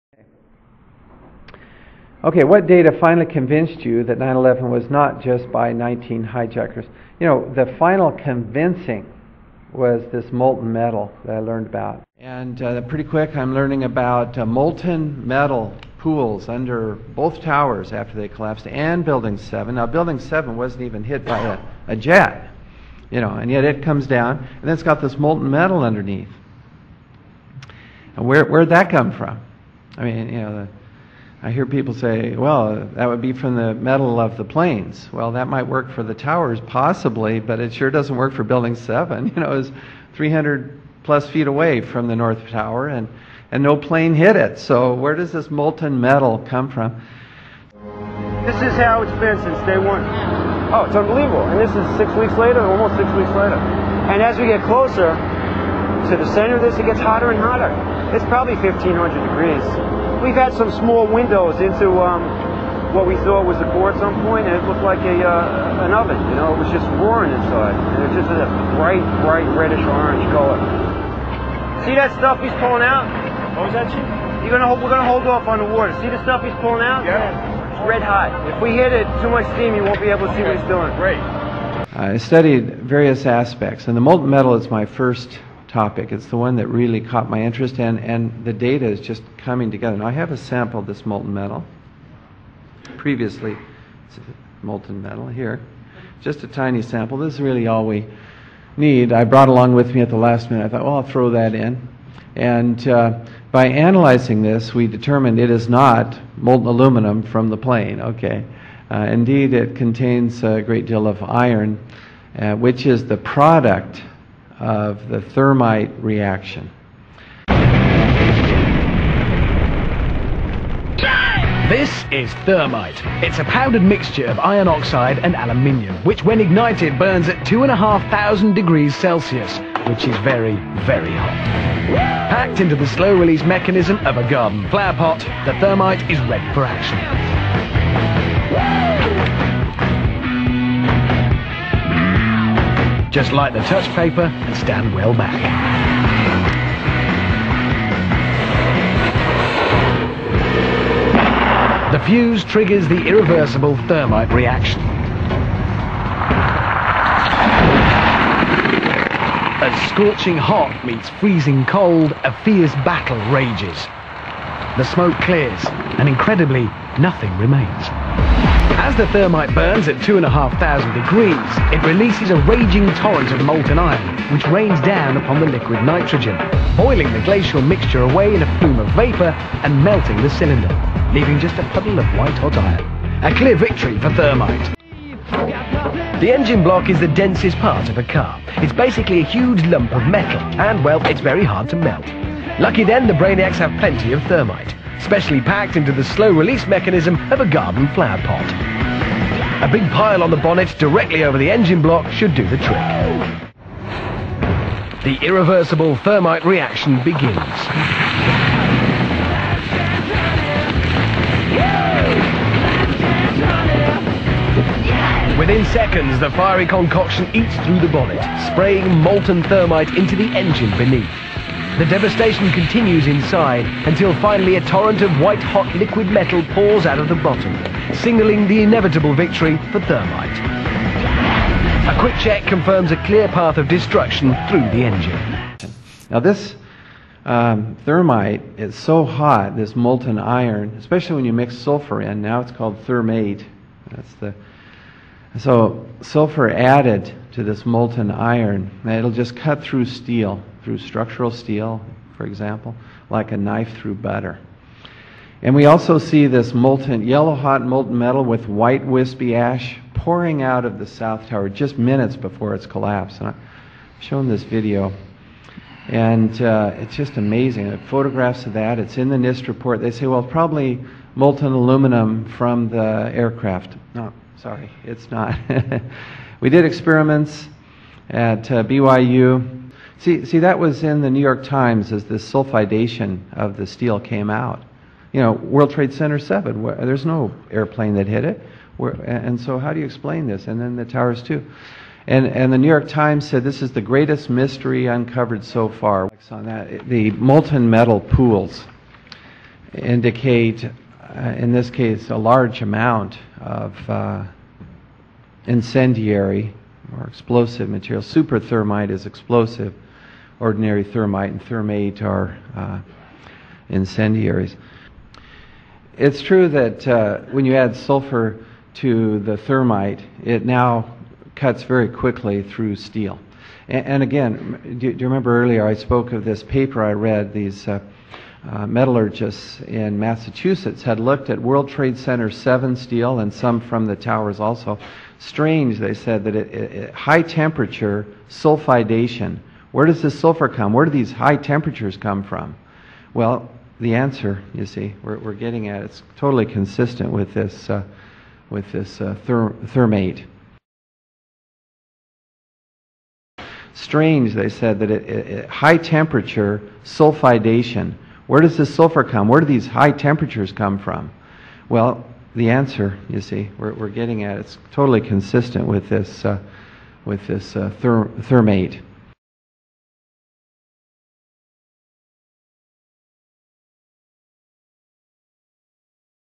Was Thermite used on 9/11 Proff Steven Jones
Why was molten metal found in  bottom of collapse site of all 3 towers?